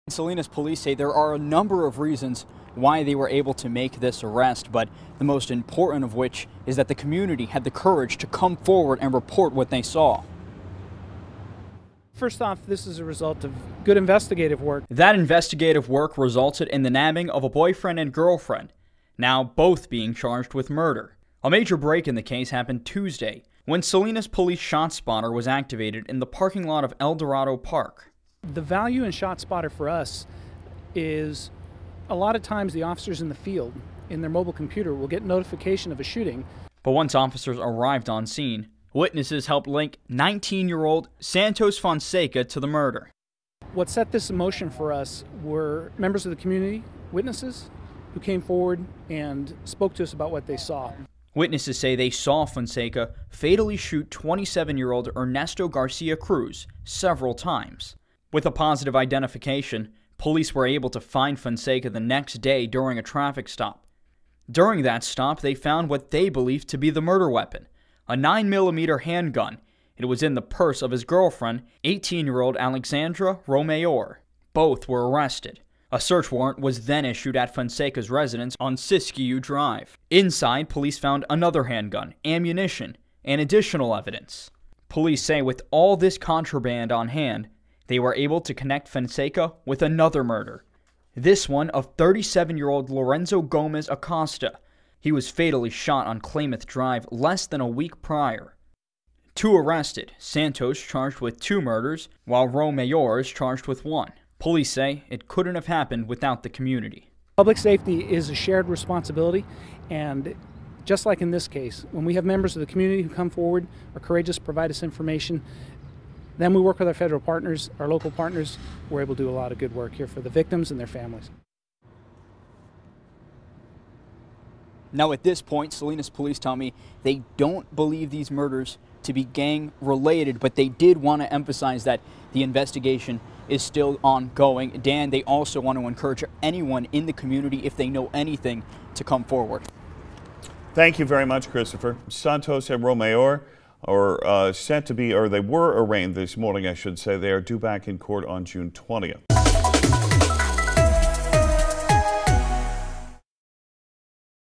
Salinas Police credits community for helping arrest couple charged with murder
Both murders happened with days of each other. Police have charged 19-year-old Santos Fonseca with 2 counts of murder and his girlfriend, 18-year-old Alekzandra Romayor, with one count of murder and another count of accessory to a murder.